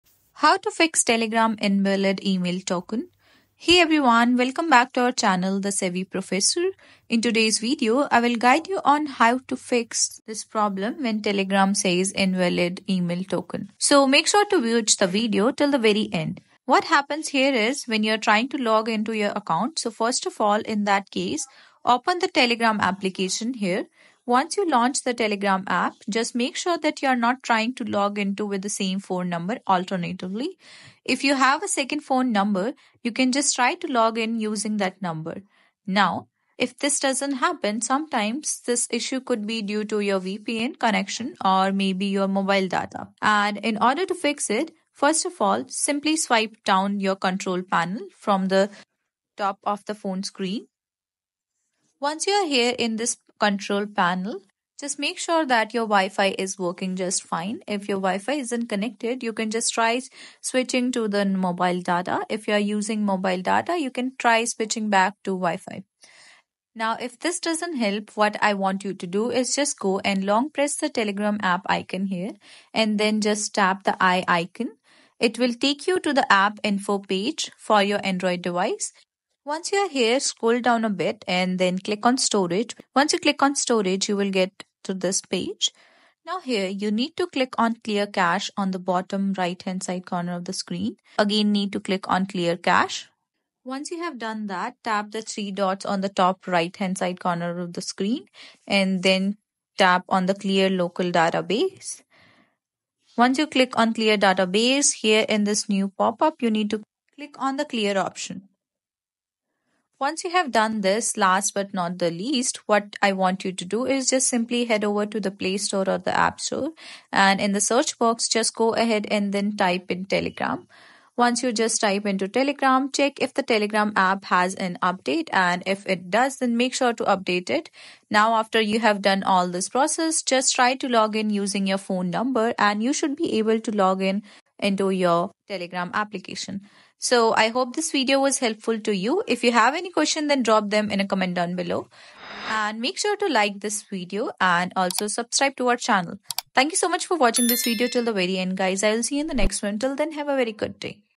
How To Fix Telegram Invalid Email Token (How To Solve Telegram Invalid Email Token)
How To Fix Telegram Invalid Email Token (How To Solve Telegram Invalid Email Token). In this video tutorial I will show you how to fix Telegram invalid email token.